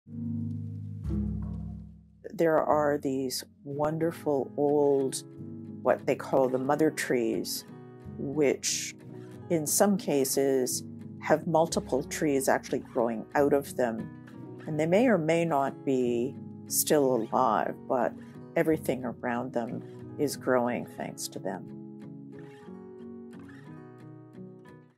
Finding the Mother Tree: Tofino (BC) Short
Finding the Mother Tree: Discovering the Wisdom of the Forest by Suzanne Simard identified how intelligent and supportive mother trees are. It was so exciting to see one in Tofino